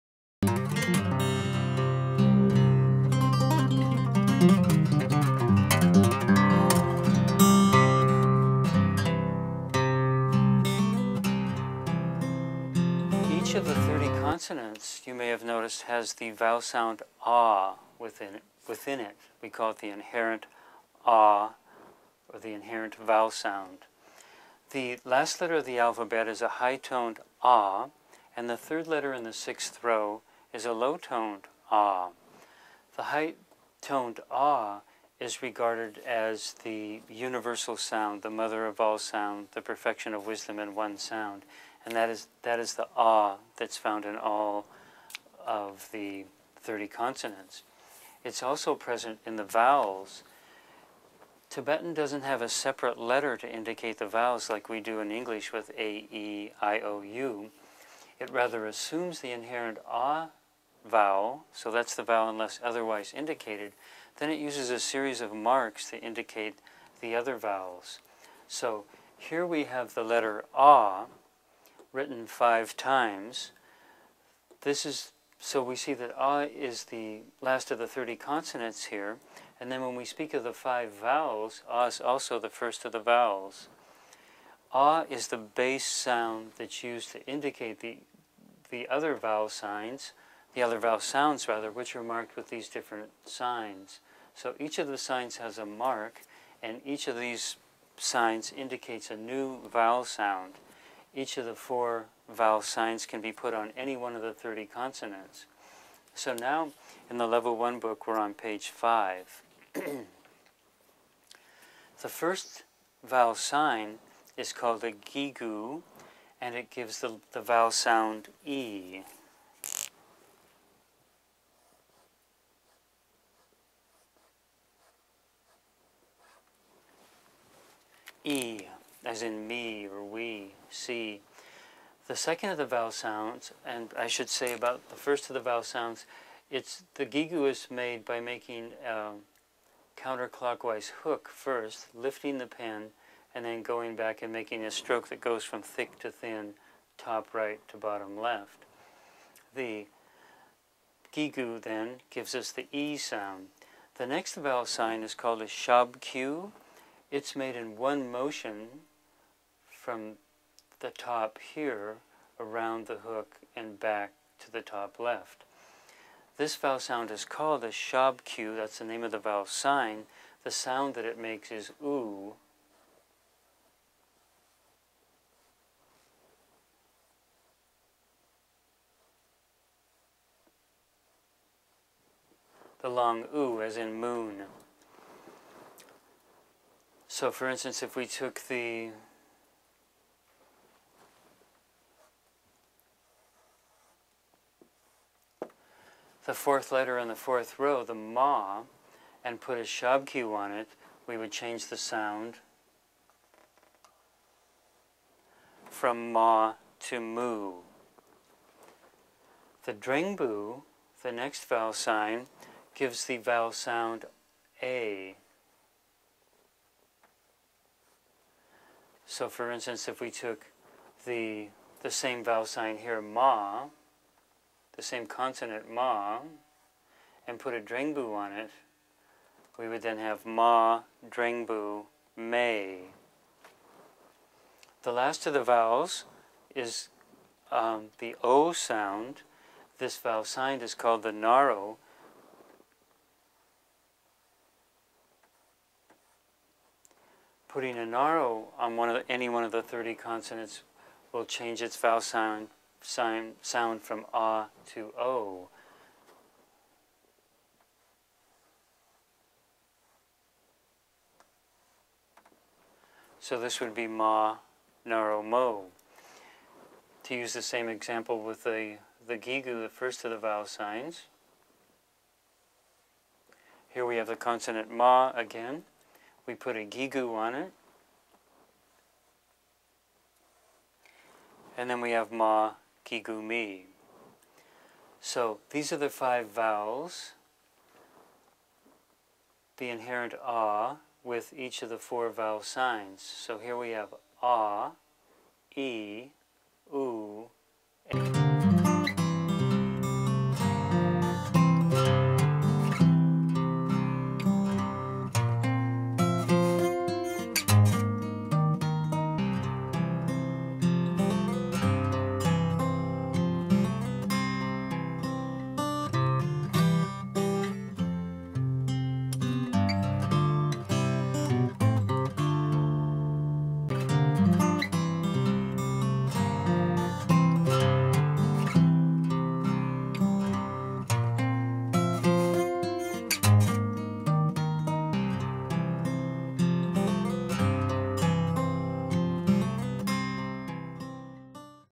Tibetan Langauge Institute: The 5 Vowels
Lama David Curtis, president of the TIbetan Language Institute explains the five Tibetan vowels: the inherent AH and the four vowels that are marked with vowel signs.